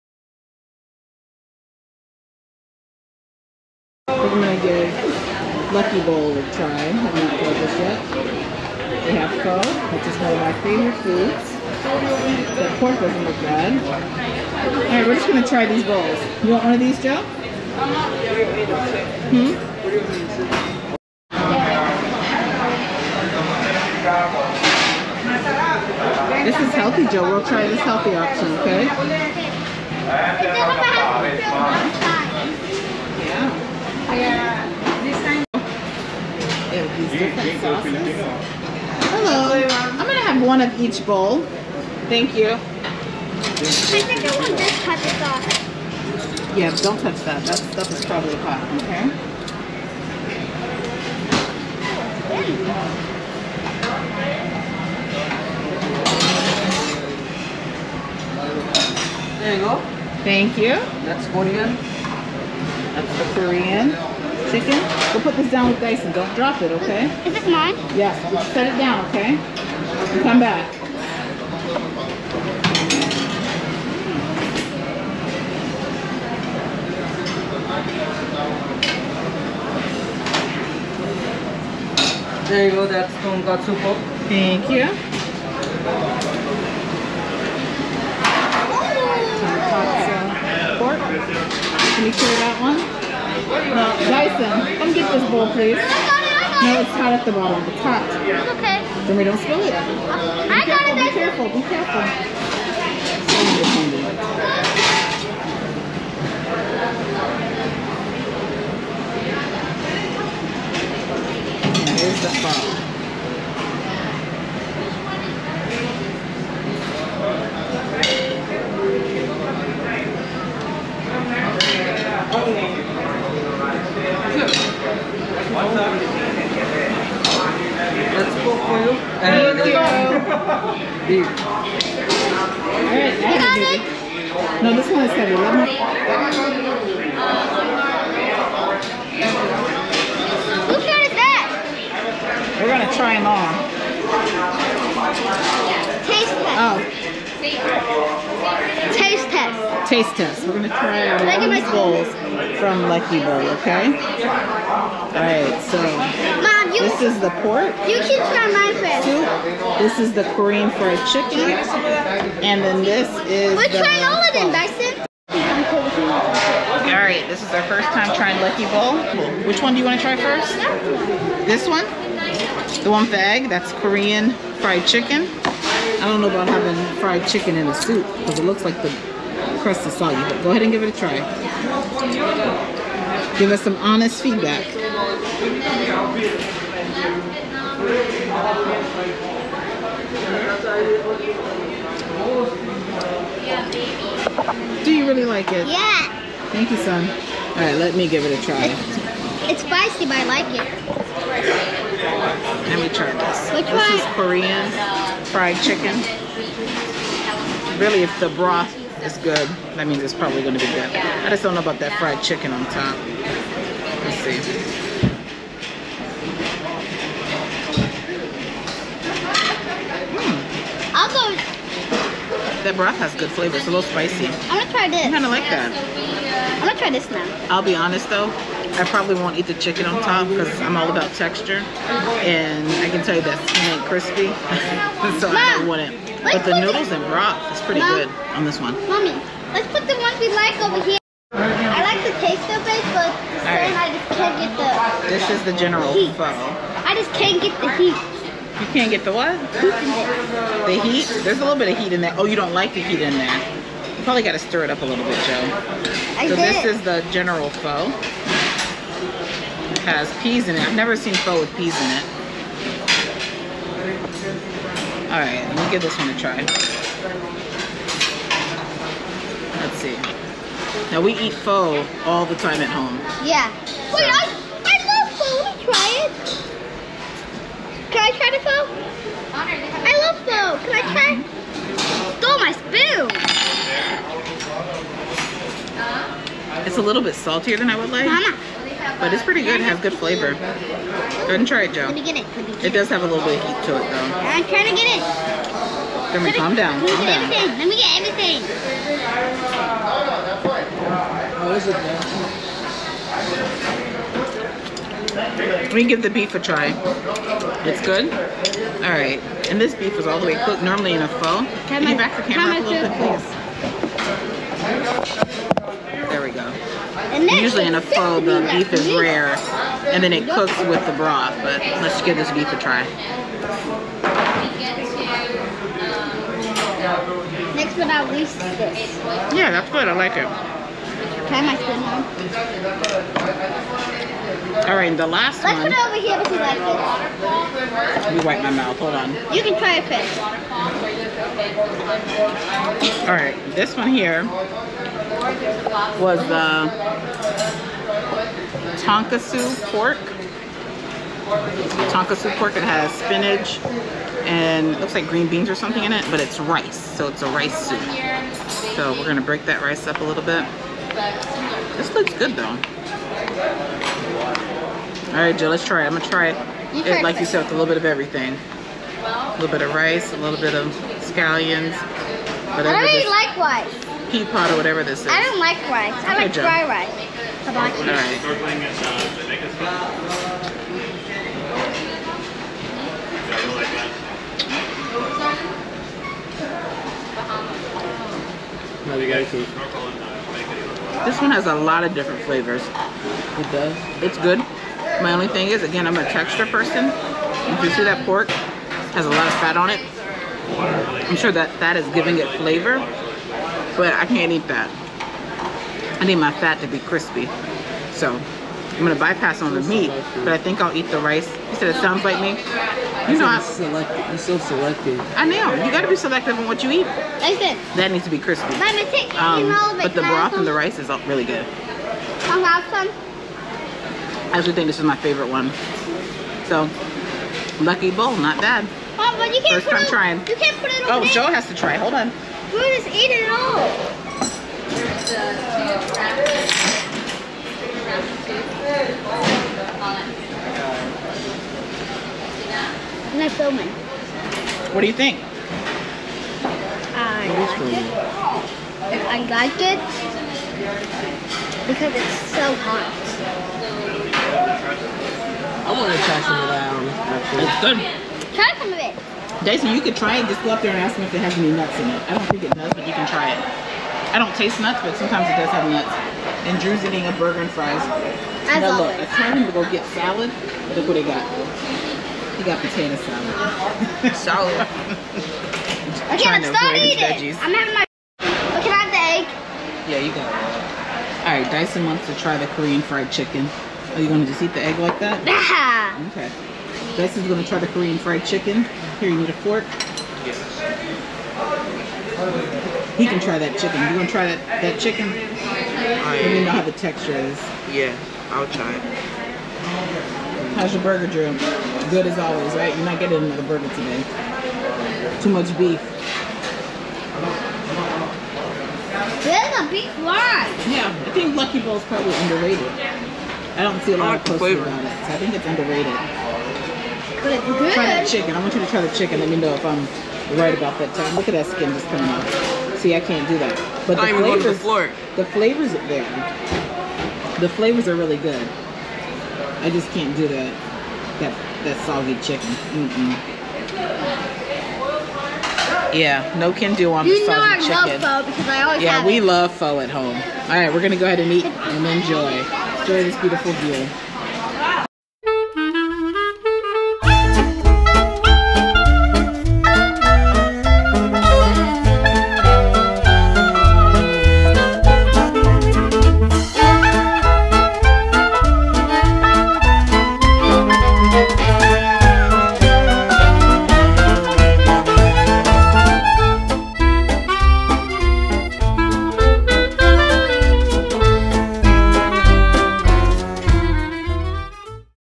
Carnival Cruise Line Lucky Bowl Restaurant Review- Is it good?
In this video we sample 3 different soups from Lucky Bowl on Carnival Sunrise. Luck Bowl is a complimentary option and is open for lunch. Lucky Bowl serves asian inspired, healthy soup bowls. Lucky Bowl is only available on Carnival Sunrise,